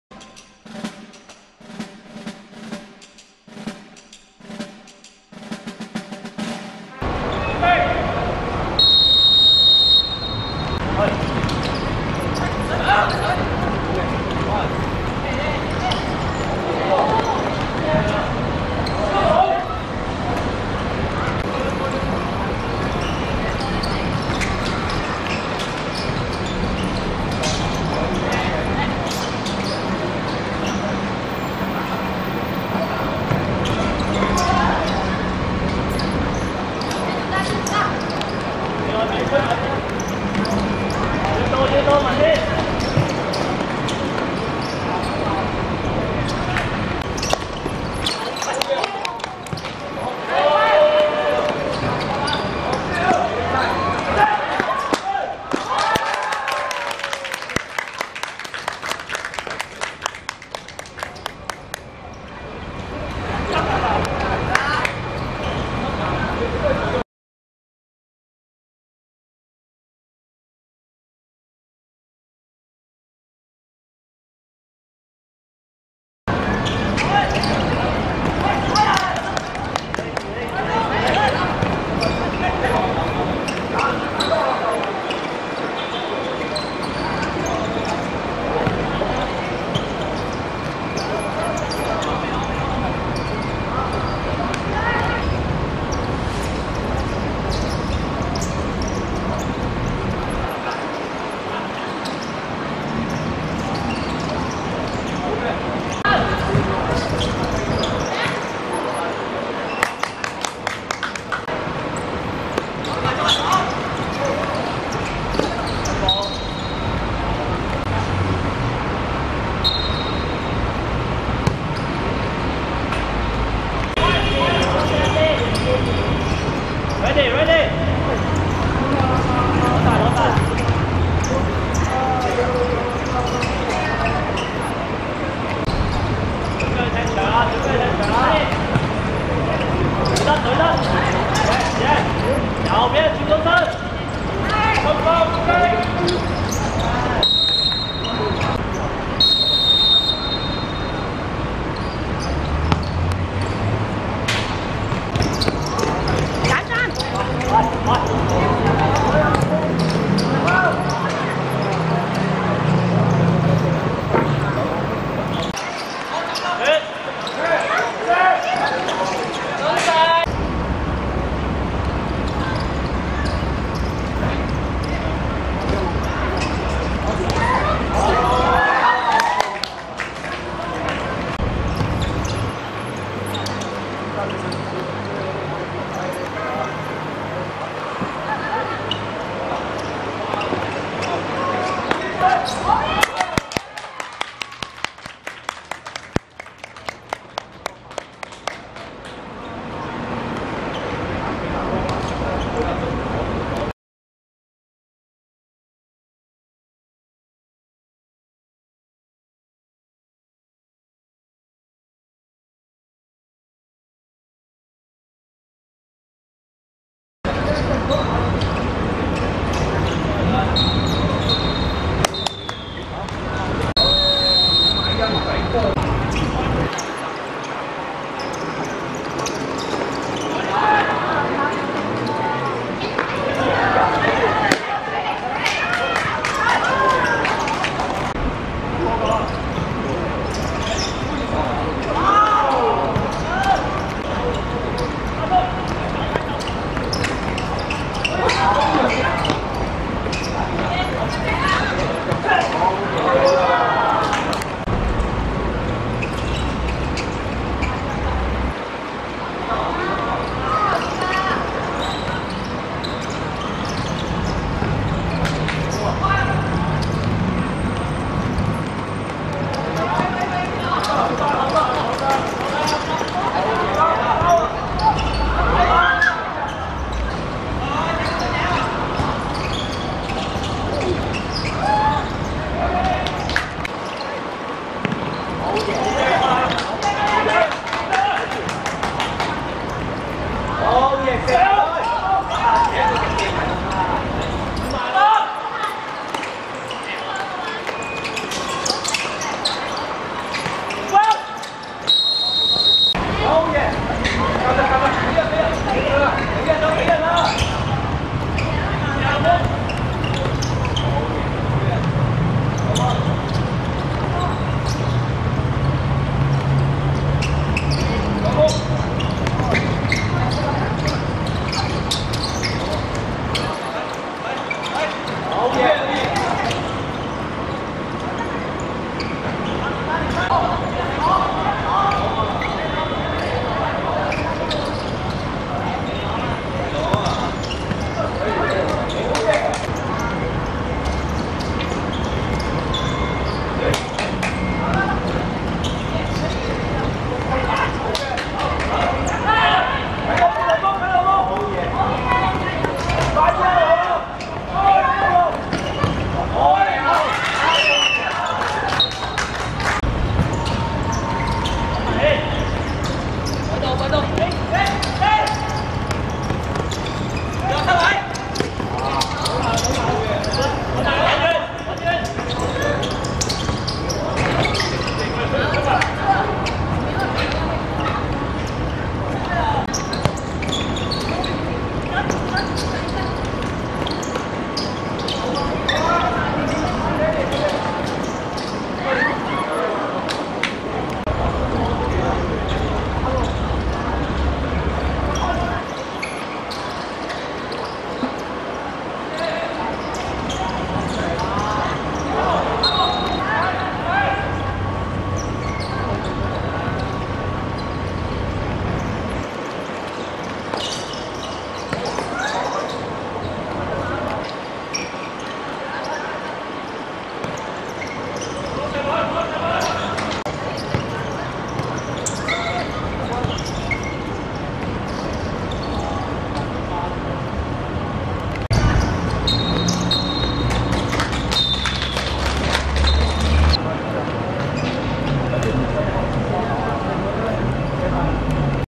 九龍城vs大魚腩(2019.9.11.九龍青少年盃U16足球賽)精華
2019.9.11.旺角麥花臣球塲 20:00 九龍城(紅) 0 : 2 大魚腩(藍)